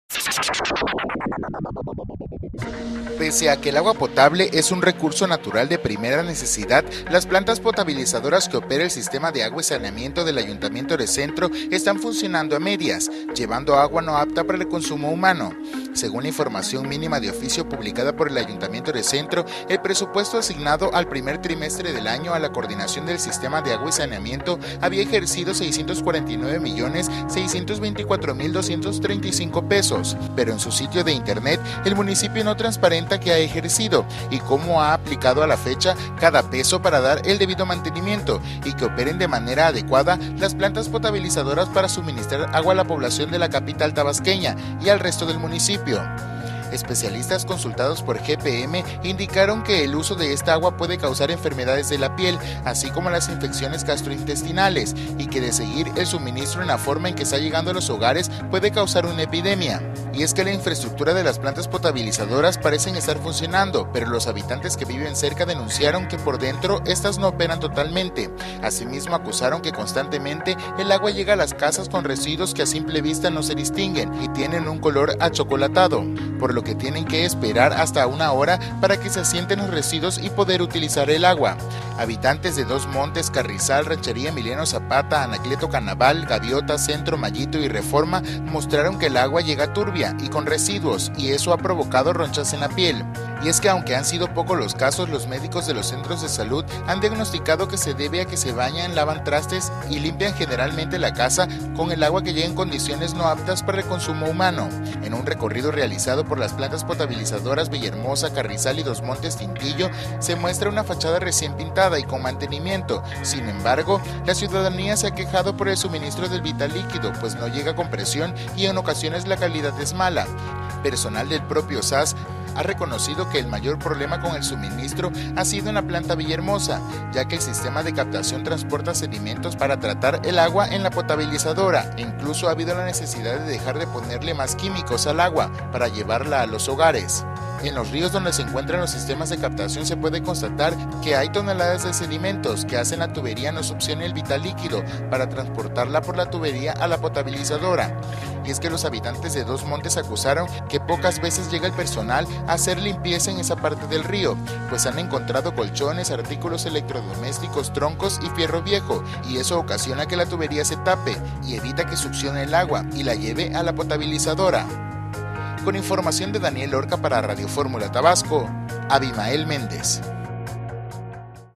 Plantas potabilizadoras del SAS Tabasco funcionan "a medias"
Pese a que el agua potable es un recurso natural de primera necesidad, las plantas potabilizadoras que opera el Sistema de Agua y Saneamiento (SAS) del Ayuntamiento de Centro, están funcionando a “medias” llevando agua no apta para el consumo humano.

Según la información mínima de oficio publicada por el Ayuntamiento de Centro, el presupuesto asignado al primer trimestre de este año, a la Coordinación de Sistema de Agua y Saneamiento (SAS) había ejercido 649 millones 624 mil 235 pesos.